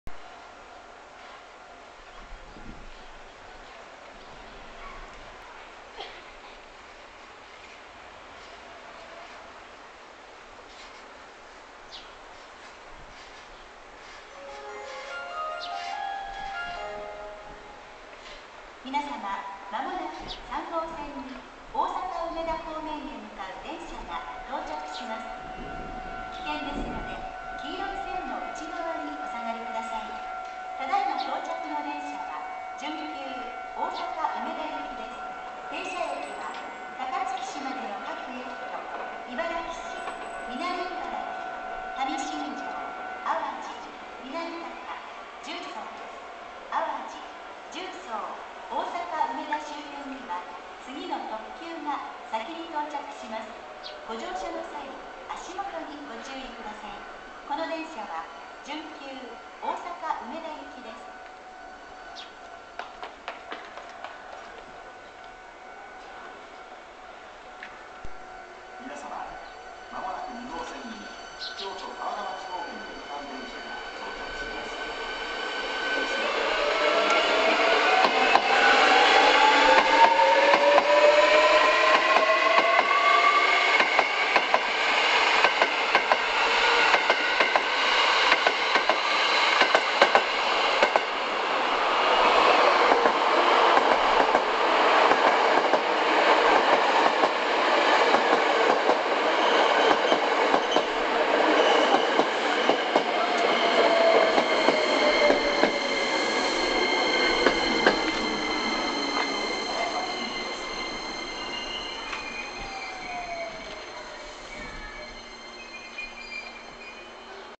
阪急電鉄 1300系 1300F 新車 営業運行2日目 準急 梅田行き 長岡天神駅にて 阪急京都線
阪急電鉄 1300系 1300F 新車  営業運行2日目 準急  梅田行き 長岡天神駅にて